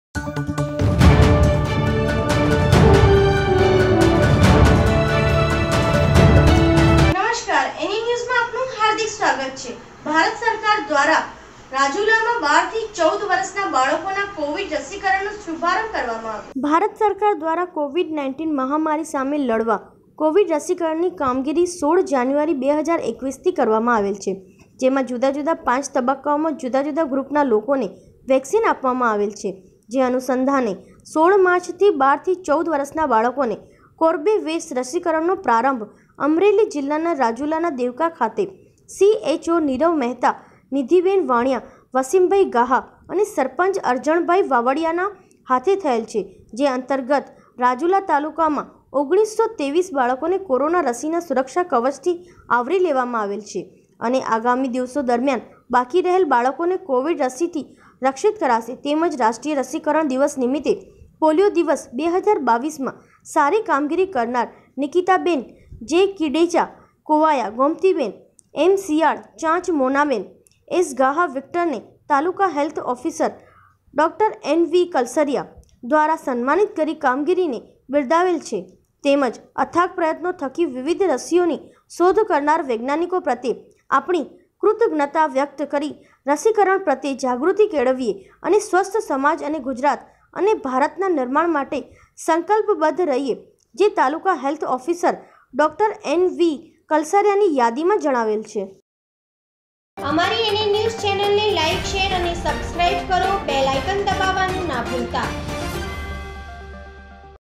રાજુલામાં ૧૨ થી ૧૪ વર્ષના બાળકોના કોવિડ રસીકરણનો શુભારંભ ...
ભારત સરકાર દ્વારા કોવિડ-૧૯ મહામારી સામે લડવા કોવિડ રસીકરણની કામગીરી ૧૬ જાન્યુઆરી ૨૦૨૧થી કરવામાં આવેલ છે જેમા જુદા-જુદા પાંચ તબક્કાઓમાં જુદા જુદા ગ્રુપના લોકોને વેક્સિન આપવામાં આવેલ જે અનુસંધાને ૧૬ માર્ચથી ૧૨ થી ૧૪ વર્ષના બાળકોને કોર્બેવેક્ષ રસીકરણનો પ્રારંભ અમરેલી જીલ્લાના રાજુલાના દેવકા ખાતે સીએચઓ નીરવ મહેતા,નિધિબેન વાણીયા,વસીમભાઈ ગાહા અને સરપંચ અરજણભાઈ વાવડીયાના હાથે થયેલ જે અંતર્ગત આજરોજ રાજુલા તાલુકામાં ૧૯૨૩ બાળકોને કોરોના રસીના સુરક્ષાકવચથી આવરી લેવામાં આવેલ અને આગામી દિવસો દરમિયાન બાકી રહેલ બાળકોને કોવિડ રસીથી રક્ષિત કરાશે તેમજ રાષ્ટ્રીય રસીકરણ દિવસ નિમિત્તે પોલિયો દિવસ-૨૦૨૨મા સારી કામગીરી કરનાર નિકીતાબેન જે.કિડેચા-કોવાયા,ગોમતીબેન એમ.શિયાળ-ચાંચ,મોનાબેન એસ.ગાહા-વિક્ટરને તાલુકા હેલ્થ ઓફિસર ડૉ.એન.વી.કલસરીયા દ્વારા સન્માનિત કરી કામગીરીને બિરદાવેલ તેમજ અથાક પ્રયત્નો થકી વિવિધ રસીઓની શોધ કરનાર વૈજ્ઞાનિકો પ્રત્યે આપણી કૃતજ્ઞતા વ્યક્ત કરી રસીકરણ પ્રત્યે જાગૃતિ કેળવીએ અને સ્વસ્થ સમાજ,ગુજરાત અને ભારતના નિર્માણ માટે સંકલ્પબદ્ધ રહીએ જે તાલુકા હેલ્થ ઓફિસર ડૉ.એન.વી.કલસરીયાની યાદીમા જણાવેલ છે.
રિપોર્ટર અલારખ ગાહા રાજુલા અમરેલી